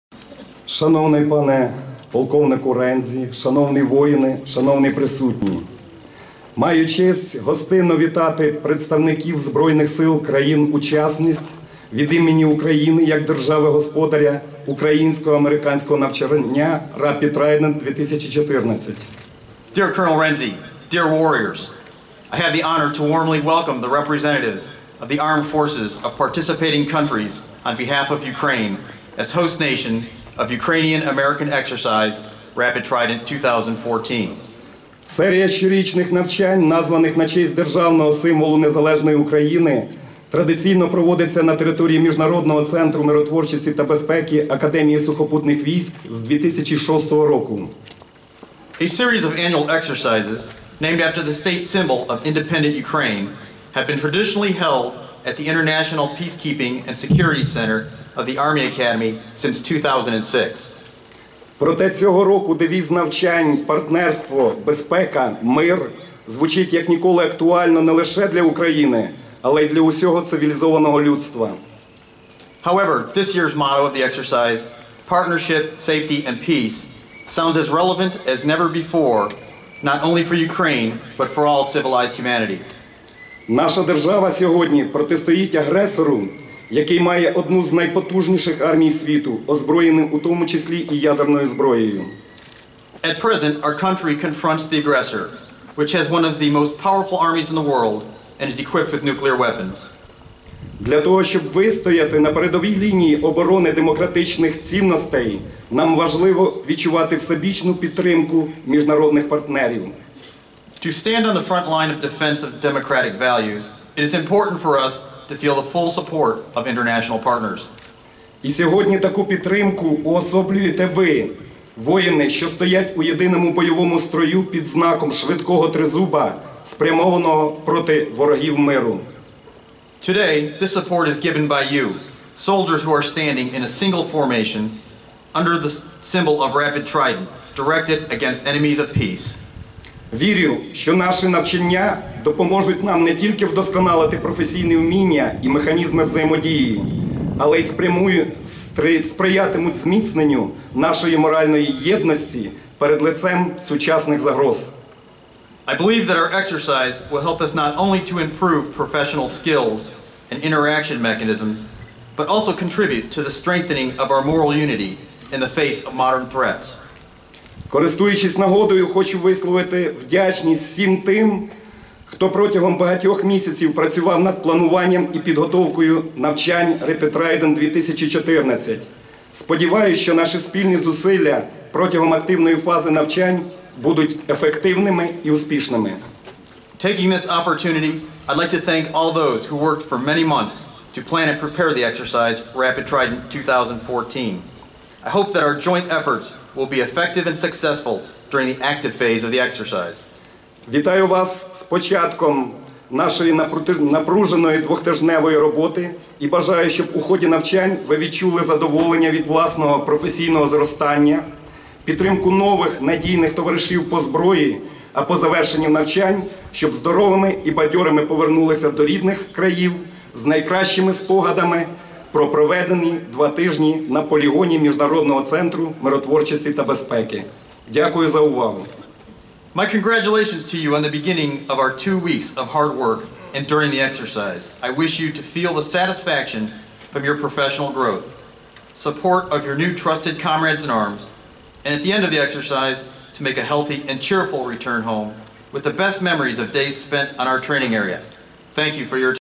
RT14 Opening Ceremony Colonel Sivak Speech
Exercise Co-Director, Ukrainian Colonel Alexander Sivak gives a speech at the opening ceremony for Rapid Trident 2014, near L'viv Ukraine.